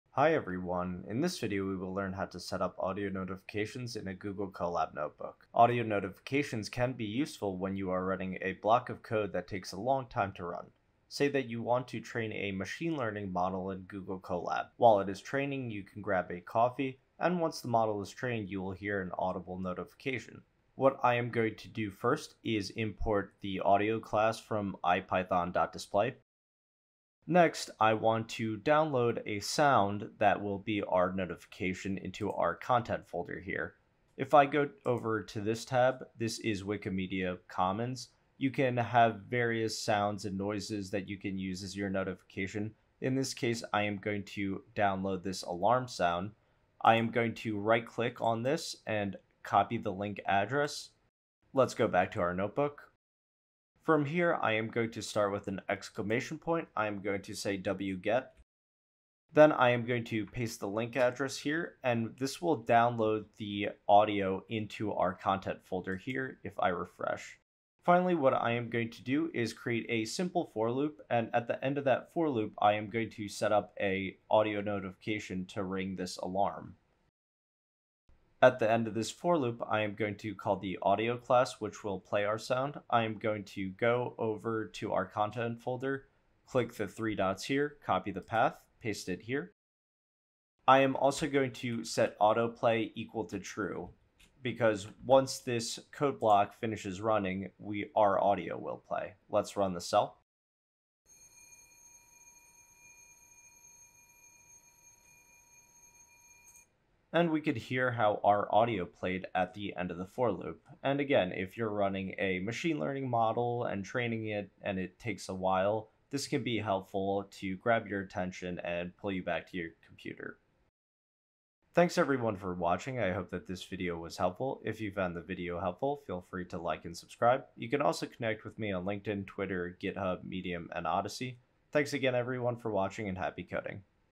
Google Colab! Audio Notifications
Tutorial on implementing an audio notification once a code block finishes running in Google Colab.

The notebook can be found in the "Google Colab Tutorials" folder within the below repo.

|-Video Chapters-|
0:00 - Intro
0:20 - Importing the Audio class
0:27 - Downloading an audio sound bite
1:07 - Setting an audio notification in a cell
2:01 - Conclusion